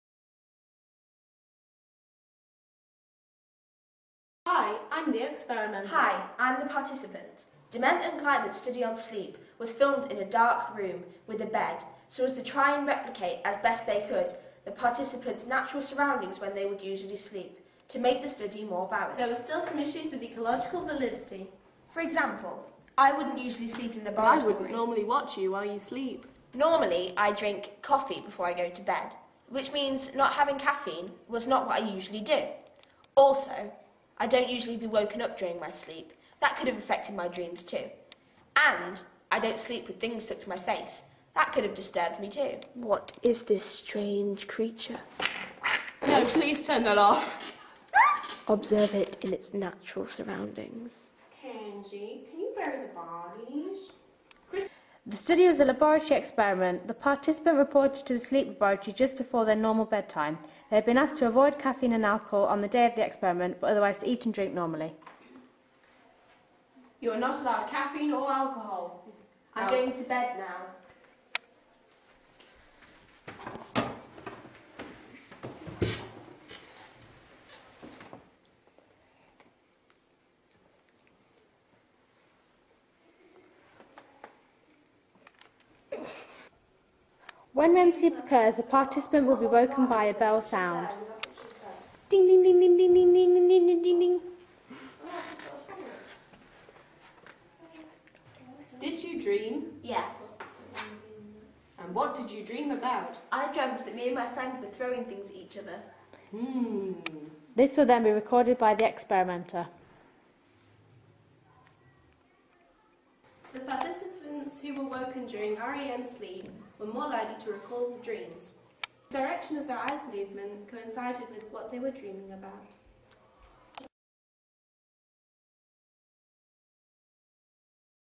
Dement and Kleitman - Study
Core studies - Physiological. Dement and Kleitman.

Students act out D&K's study.